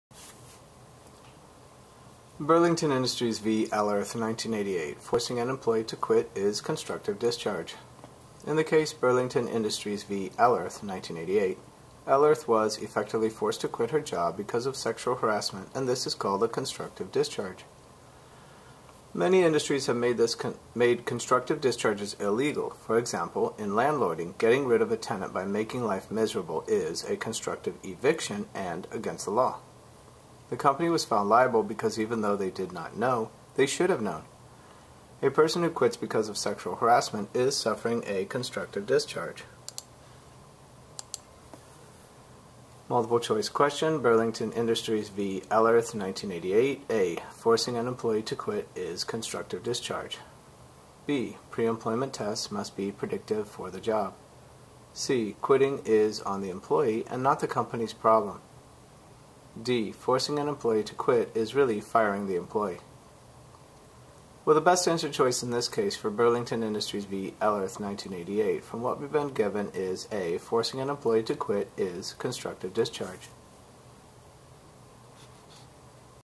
Burlington Industries v. Ellerth (1988) PHR SPHR Human Resources License Exam PHRPass.com
Burlington Industries v. Ellerth (1988)

forcing an employee to quit is constructive discharge

In the case Burlington Industries v. Ellerth (1988), Ellerth was effectively forced to quit her job because of sexual harassment and this is called a “constructive discharge.” Many industries have made constructive discharges illegal. For example in landlording, getting rid of a tenant by making life miserable is a constructive eviction and against the law. The company was found liable because even though they did not know, they should have known. A person who quits because of sexual harassment is suffering a constructive discharge.

A forcing an employee to quit is constructive discharge
B pre-employment tests must be predictive for the job
C quitting is on the employee and not the company’s problem
D forcing an employee to quit is really firing the employee

Correct Answer: a

PHR and SPHR are registered trademarks of Human Resources Certification Institute

Find links to all our playlists at PHRPass.com